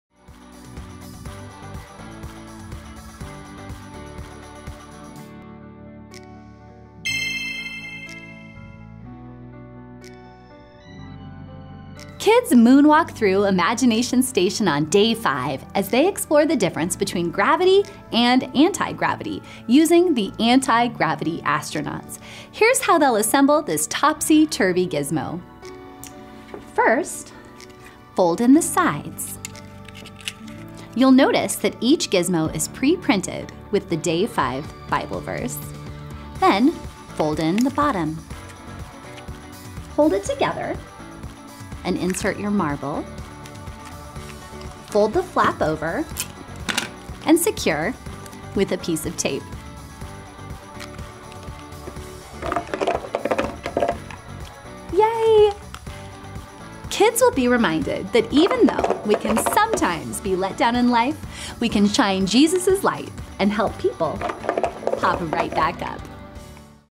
Anti Gravity Astronauts | Imagination Station
See how to make the Day 5 Sciency-Fun Gizmo roll into action at Stellar VBS!